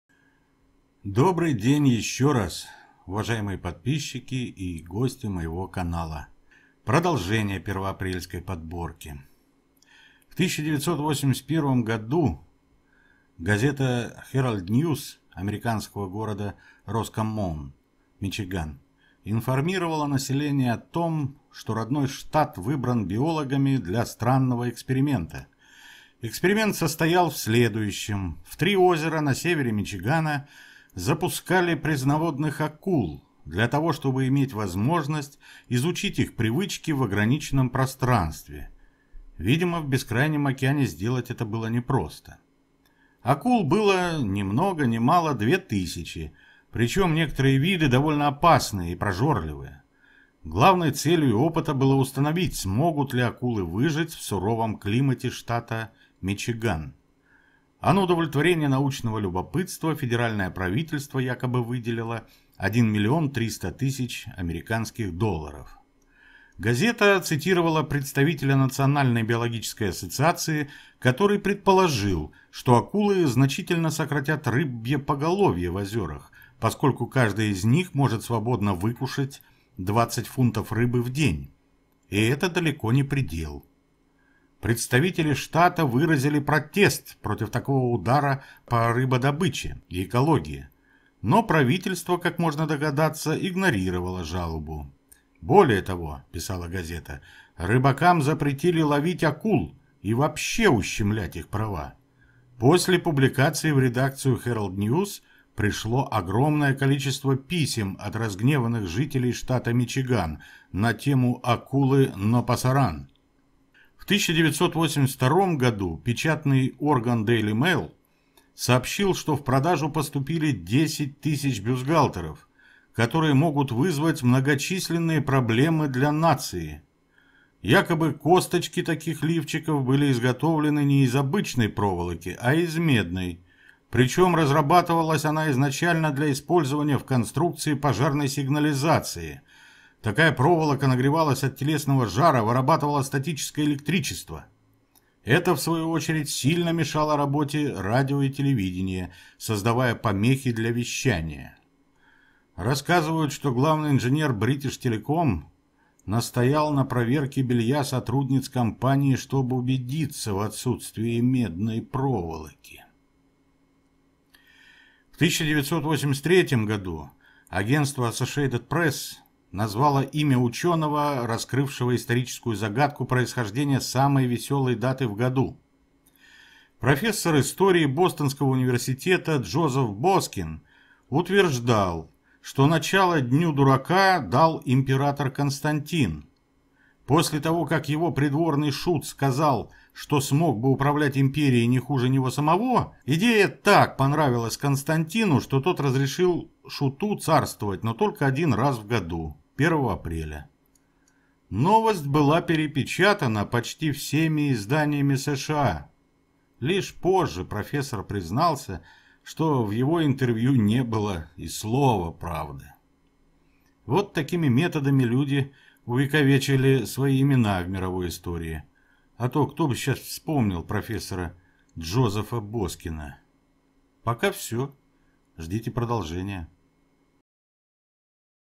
Топ подборка6 самых смешных и не очень розыгрышей на День Дурака в мире
В 1981 году газета Herald - News американского города Роскоммон, Мичиган информировала население о том, что родной штат выбран биологами для странного эксперимента. Эксперимент состоял в следующем: в 3 озера на севере Мичигана запускали пресноводных акул для того, чтобы иметь возможность изучить их привычки в ограниченном пространстве (видимо, в бескрайнем океане сделать это было непросто). Акул было, не много не мало, 2 тысячи, причем некоторые виды – довольно опасные и прожорливые. Главной целью опыта было установить, смогут ли акулы выжить в суровом климате штата Мичиган, а на удовлетворение научного любопытства федеральное правительство якобы выделило 1 миллион 300 тысяч долларов. Газета цитировала представителя Национальной Биологической Ассоциации, который предположил, что акулы значительно сократят рыбье поголовье в озерах, поскольку каждая их них может свободно выкушать 20 фунтов рыбы в день, и это далеко не предел. Представители штата выразили протест против такого удара по рыбодобыче и экологии, но правительство, как можно догадаться, игнорировало жалобу. Более того, писала газета, рыбакам запретили ловить акул и вообще ущемлять их права. После публикации в редакцию Herald – News пришло огромное количество писем от разгневанных жителей штата Мичиган на тему «акулы no pasaran!».
В 1982 году печатный орган Daily Mail сообщил, что в продажу поступили 10 000 бюстгальтеров, которые могут вызвать многочисленные проблемы для нации. Якобы « косточки » таких лифчиков были изготовлены не из обычной проволоки, а из медной, причем разрабатывалась она изначально для использования в конструкции пожарной сигнализации. Такая проволока, нагревшись от телесного жара, вырабатывала статическое электричество. Это, в свою очередь, сильно мешало работе радио и телевидения, создавая помехи для вещания. Рассказывают, что главный инженер Бритиш Телеком настоял на проверке белья сотрудниц компании, чтобы убедиться в отсутствии медной проволоки.
В 1983 году агентство Associated Press назвало имя ученого, раскрывшего историческую загадку происхождения самой веселой даты в году. Профессор истории Бостонского Университета Джозеф Боскин утверждал, что начало Дню Дурака дал император Константин, после того, как его придворный шут сказал, что смог бы управлять империей не хуже него самого. Идея так понравилась Константину что тот разрешил шуту царствовать, но только один раз в год — 1 апреля. Новость была перепечатана почти всеми изданиями США. Лишь позже профессор признался, что в его интервью не было и слова правды.
Вот такими методами люди увековечили свои имена в мировой истории, а то кто бы сейчас вспомнил профессора Джозефа Боскина?
Пока все, ждите продолжения.